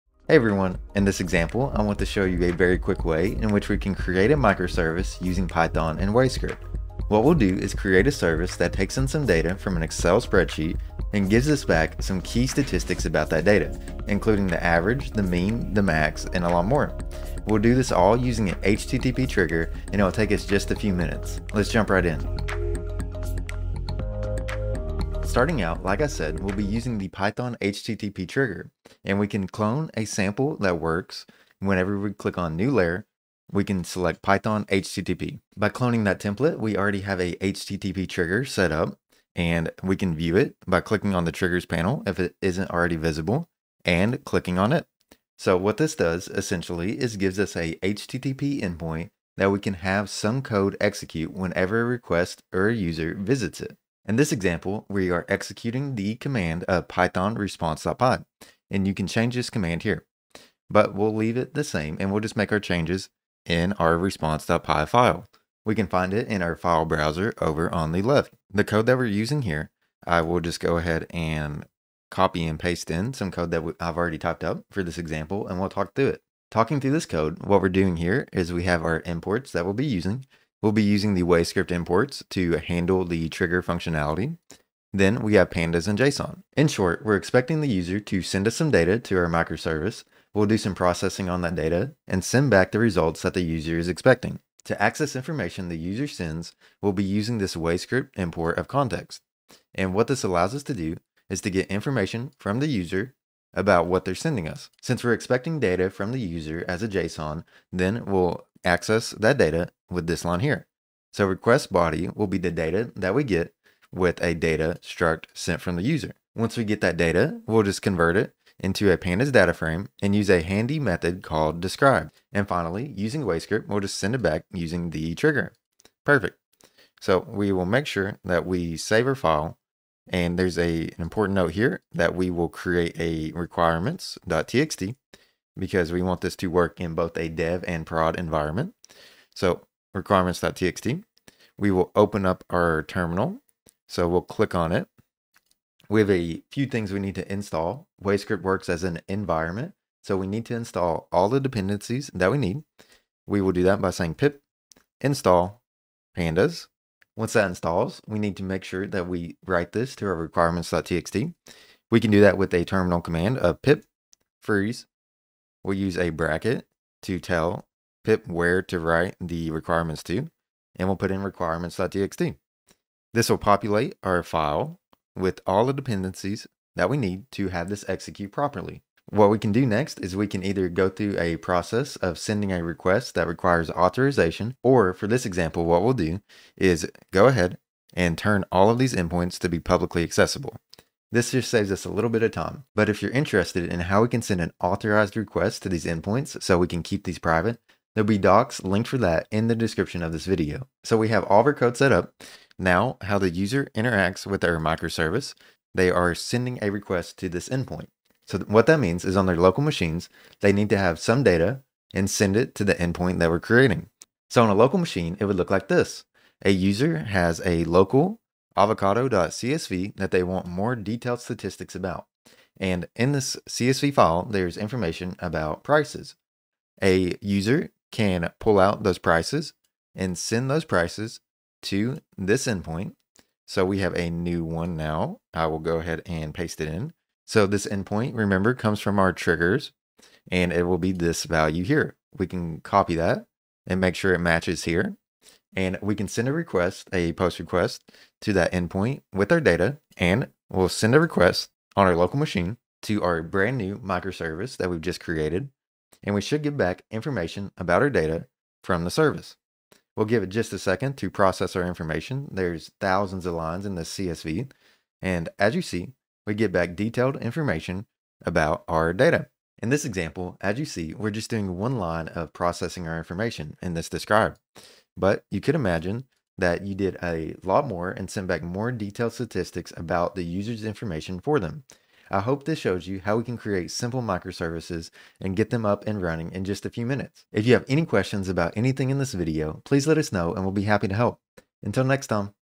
Build a Python Microservice in Minutes | Execute Code on URL visit
Using WayScript, devs rapidly build production-grade microservices and tools with unprecedented speed and security.
➤ TIMESTAMPS

Start shipping internal tools that make a difference today. Visit us at wayscript.com.